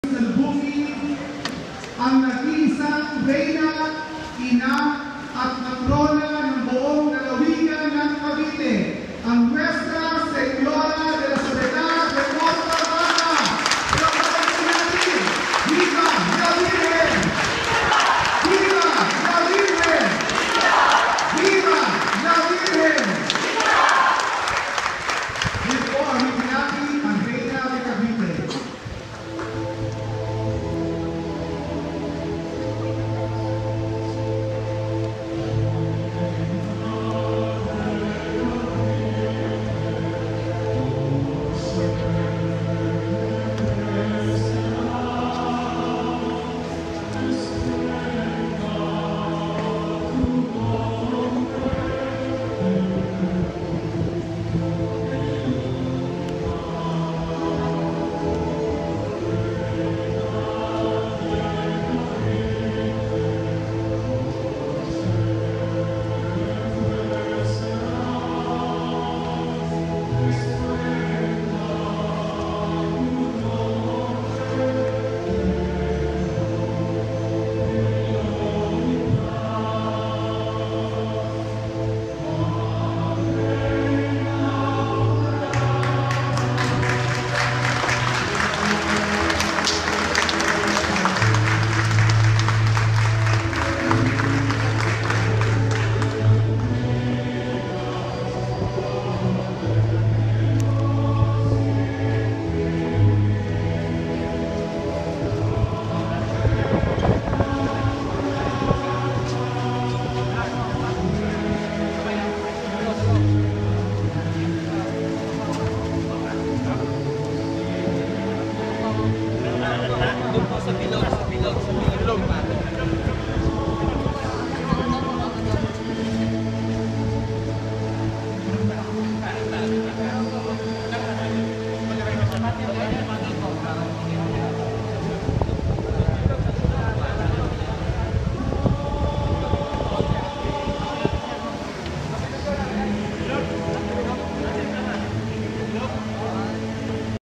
DALAW SOLEDAD 2014: Bacoor Poblacion (5)
DALAW SOLEDAD 2014:
(Part 5) Ang pagpasok ng Reina de Cavite sa Simbahan ng San Miguel, Poblacion, Bacoor city, Cavite.